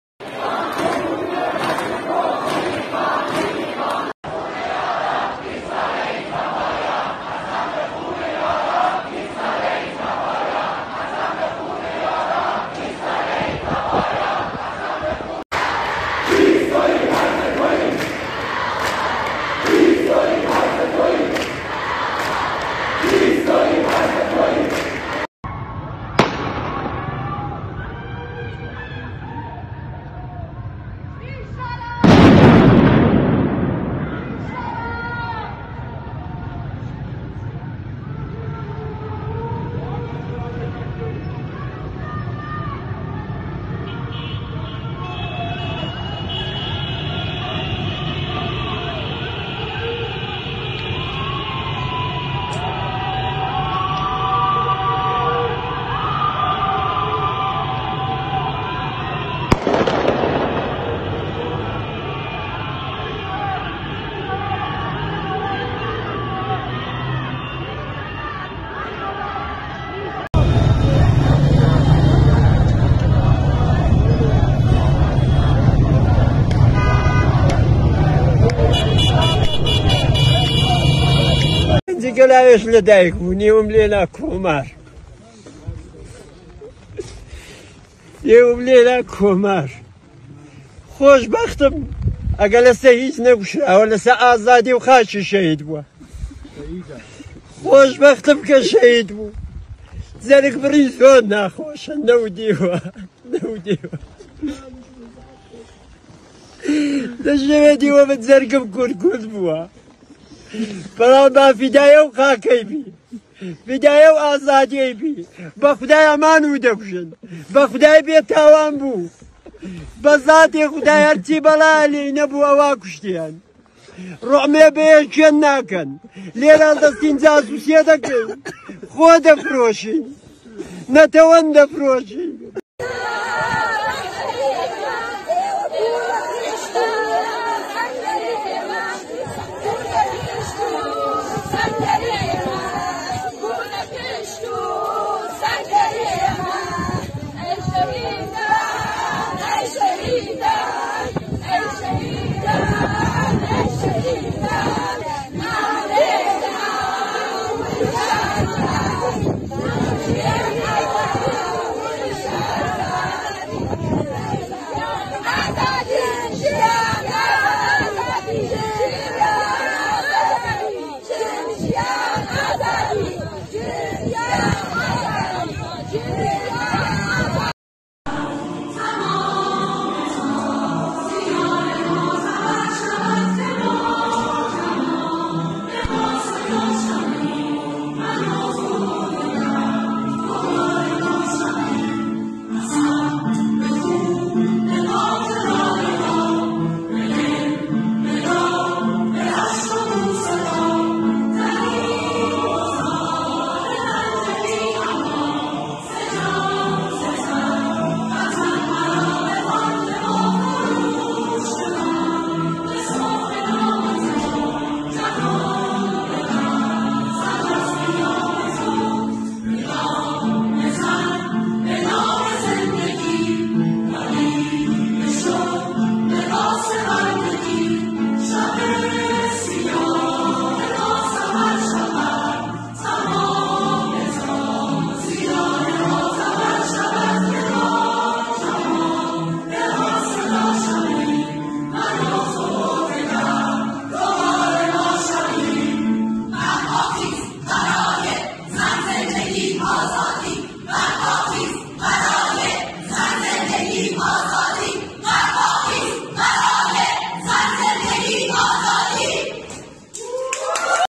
İran’da protestolar sürüyor: Çer çöp sensin, özgür kadın benim!
PİRHA- İran’da devam eden protestolar, üniversitelerden sokağa devam ediyor. İran rejim güçleri ise protestoları kanla bastırmaya çalışıyor.